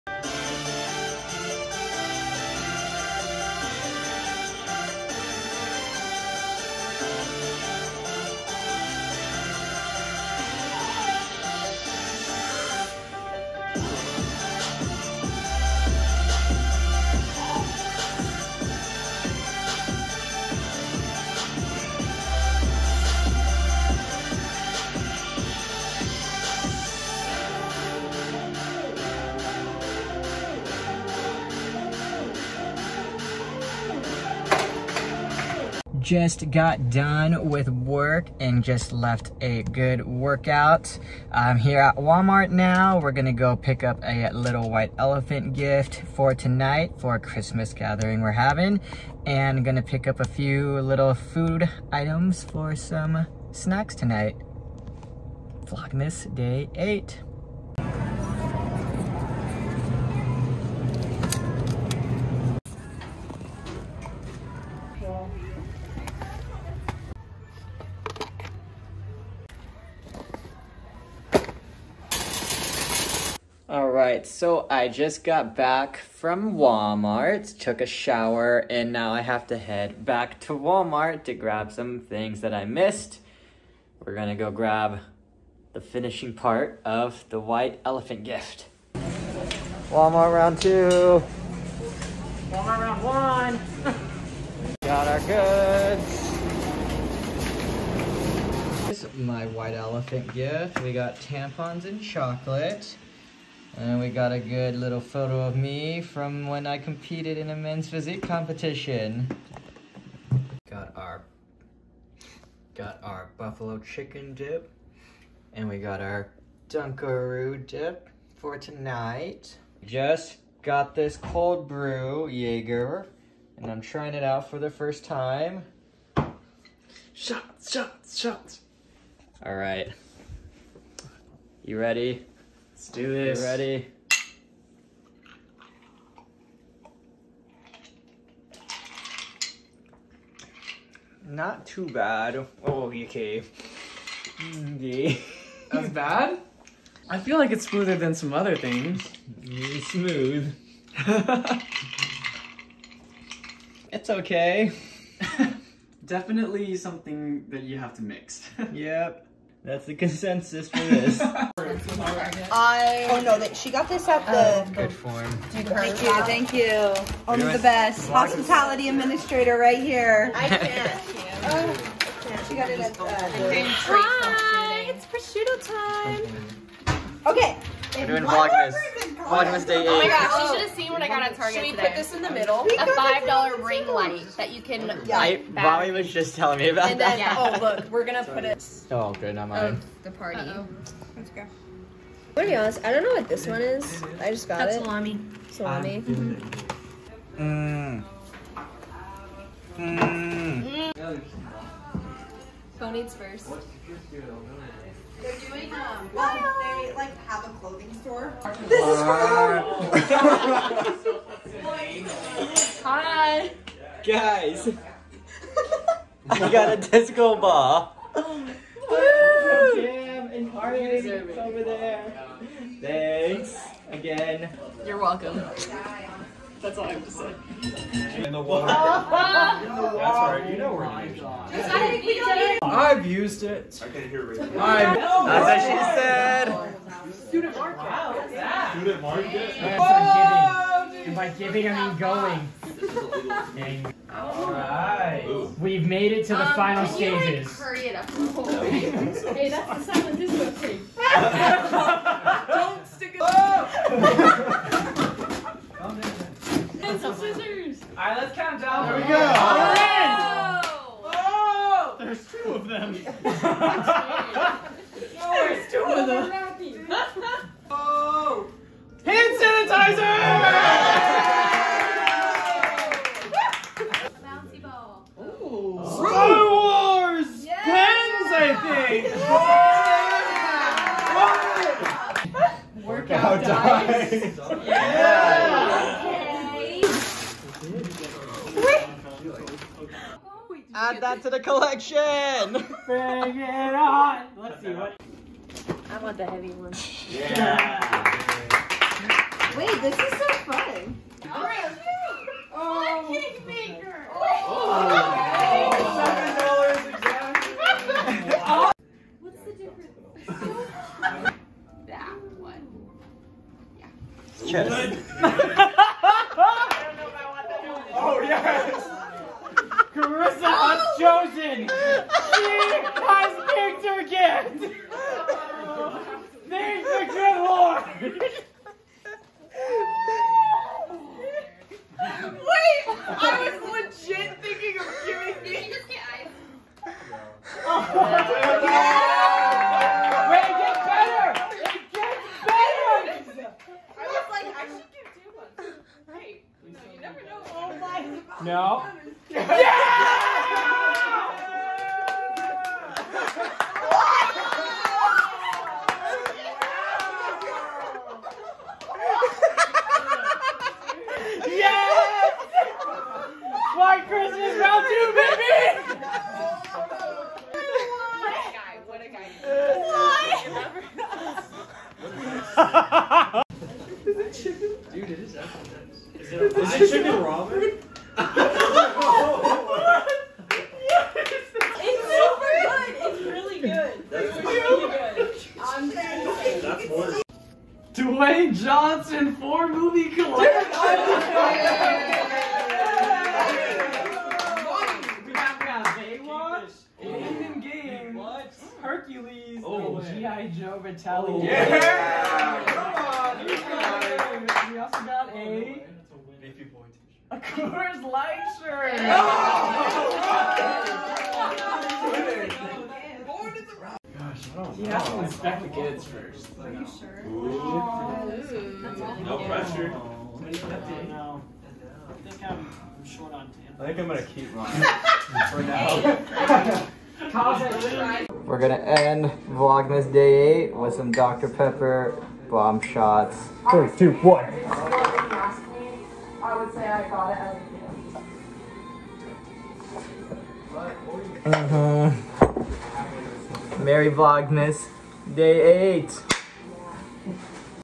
My White Elephant Gift (Vlogmas: Day 8)
Vlogmas Day 8:

Went to work, went shopping for a white elephant gift exchange and had people over for the night. What's your go to white elephant/gag gift?